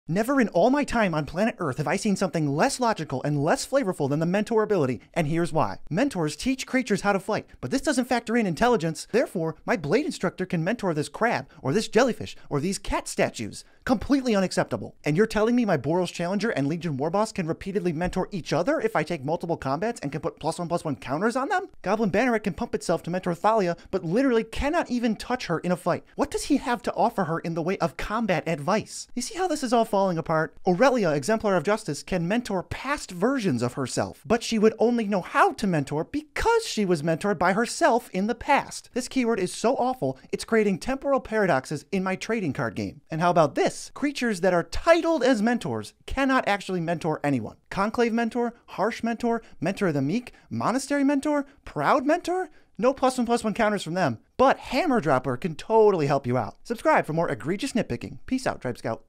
Mentor Makes NO Sense | Magic the Gathering
Here's everything wrong with the Mentor mechanic that keeps me awake at night!

Twitter.com/NitpickingNerds

Another weekend, another release! This one is all about the Mentor mechanic that Boros has utilized in the past. Turns out, it makes no sense flavorfully, so I'm going to pick it apart in a way that only the Nitpicking Nerds can! Hope you enjoy this Mentor flavor fails video!